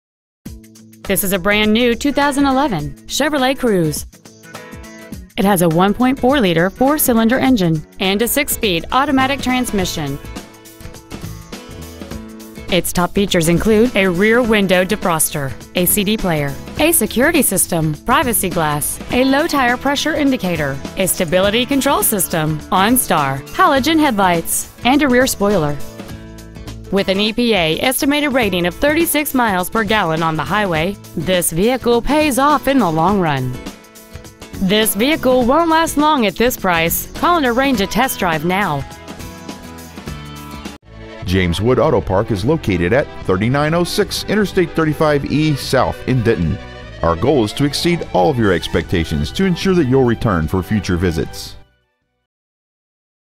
2011 Chevrolet Cruze Denton TX
Year : 2011
 Make : Chevrolet
 Model : Cruze
 Engine : 1.4L 4 Cyl. Sequential-Port F.I.
 Trans . : 6 Speed Automatic
 Exterior : Black Granite Metallic
 Miles : 98

 Stock : 214000

 2011 Chevrolet Cruze Denton TX